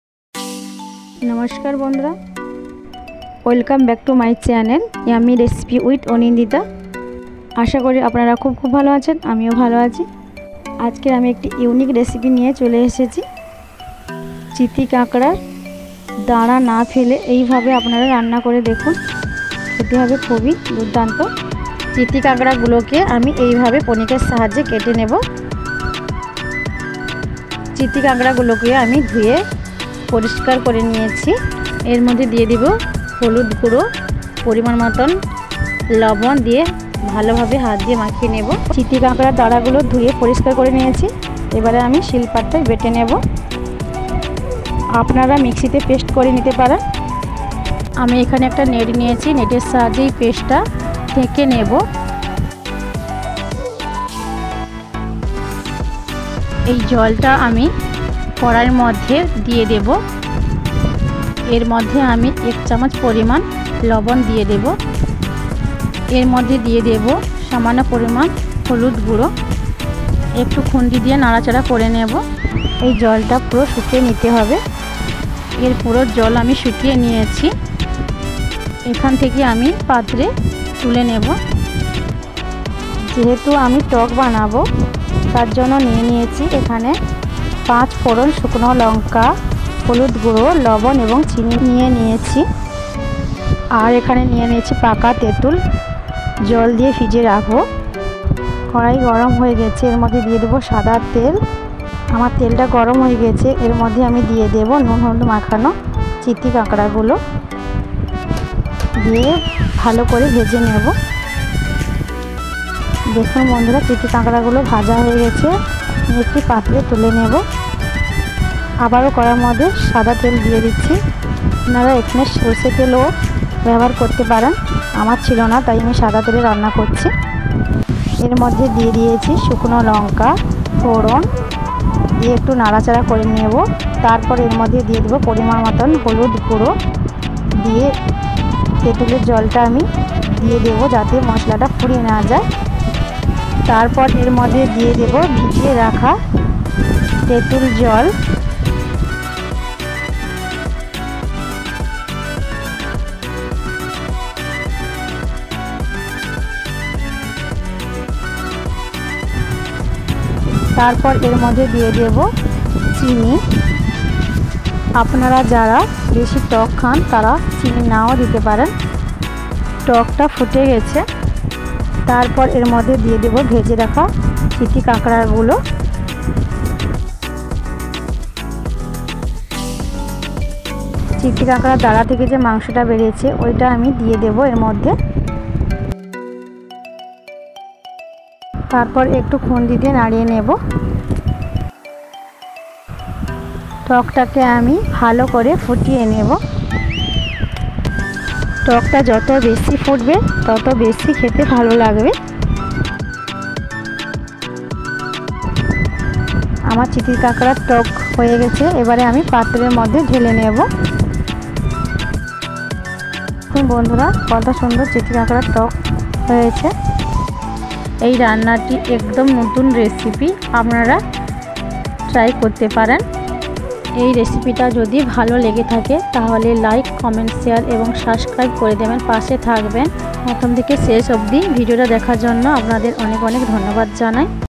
🦀চিতি কাঁকড়া🦀 দাঁড়া না ফেলে এইভাবে রান্না করুন খেতে হবে খুবই দুর্দান্ত🤤।🦀Chiti Kakra🦀 Tok Recipe।
Today I tried a new  recipe with Chiti kakra stand For the first time Yummy Recipe With Anindita channel। Cook the leopard crab like this with out leaving it standing and it will be delicious and wonderful।Tetul Sour recipe with cheetha crab stand।
 This recipe is a very simple recipe।
   Thank you very all very much।
 Ingredients:-
 chitikakra।
cooking oil।
salt to taste।
 turmeric powder।
 suger।
pachforon।
 dry Chili।
water।
bitter gourd।